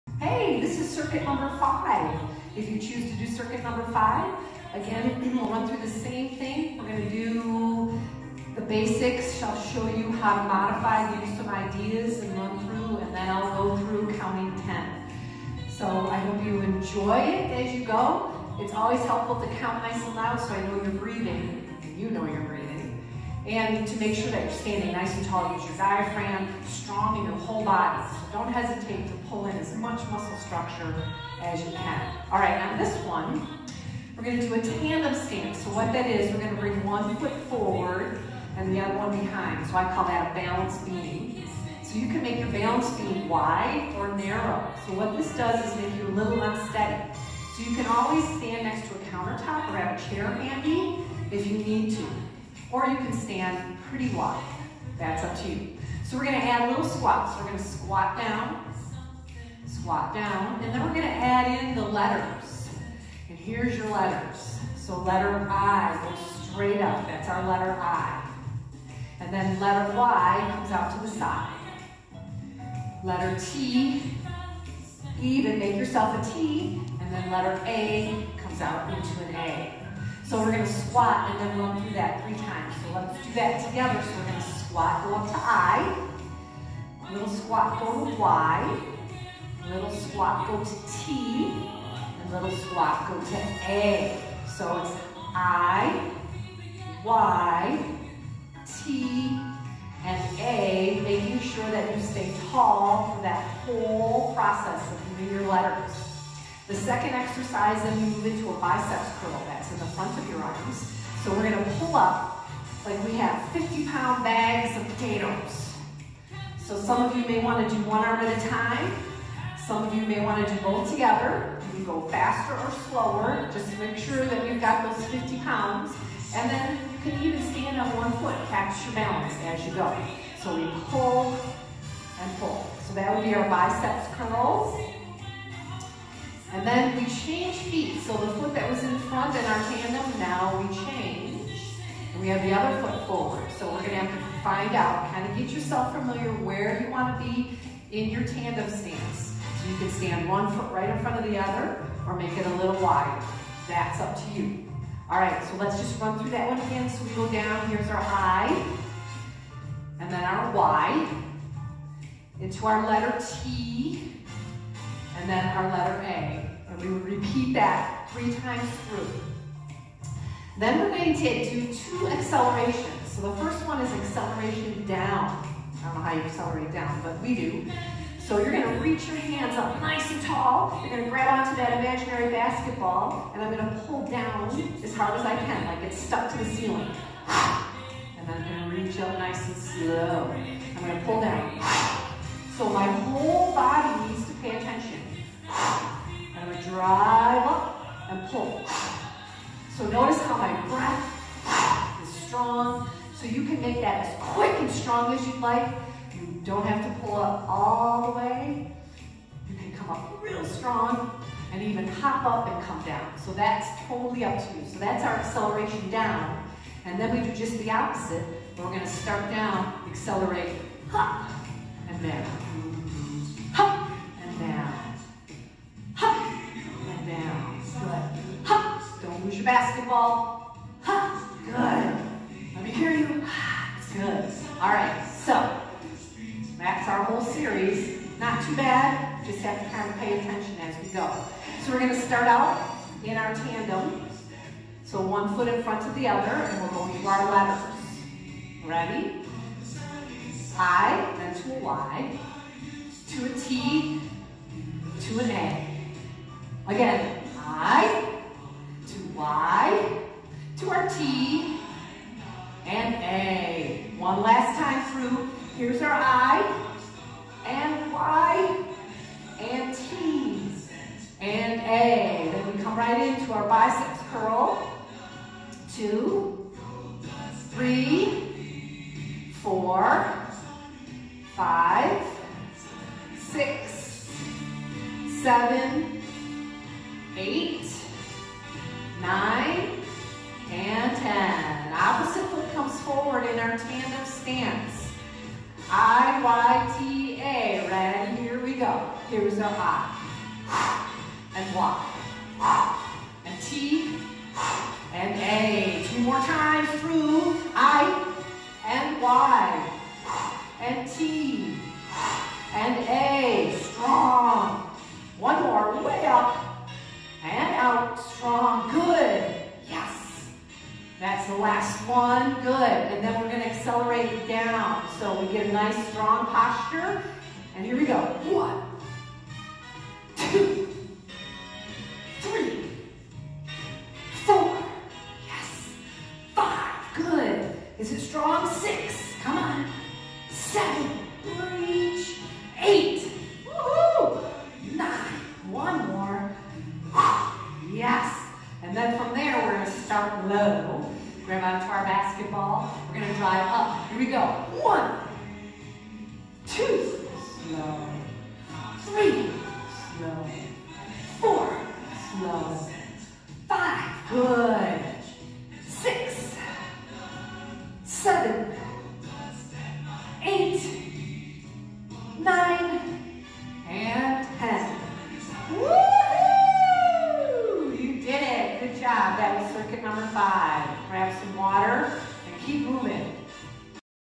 Active Aging Circuit #5
Active Aging entry level workout with Suzanne!